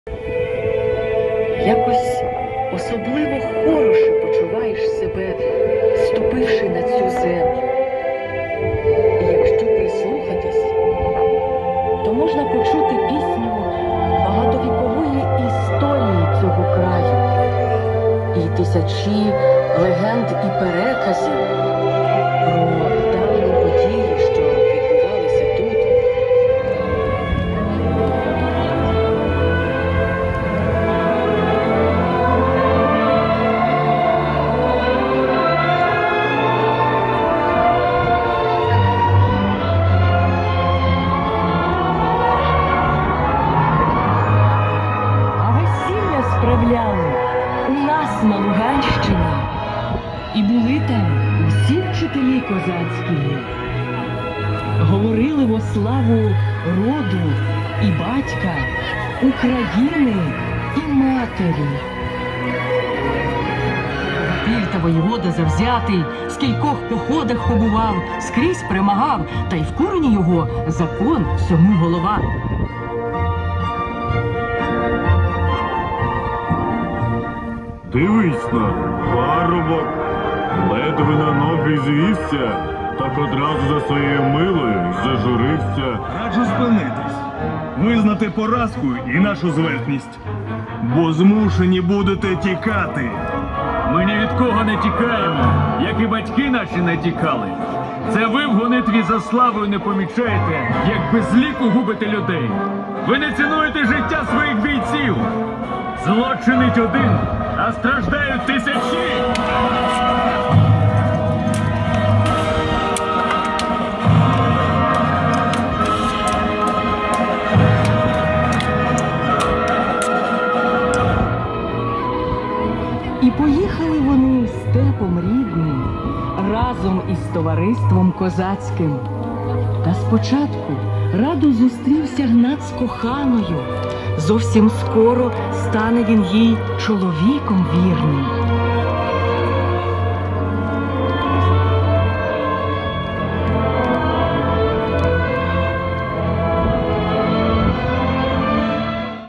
Поема про землю Луганську
Театралізована композиція з елементами історичної реконструкції «Поема про землю Луганську» від Луганського обласного академічного українського музично-драматичного театру на горі біля Козацької вежі у місті Сватове.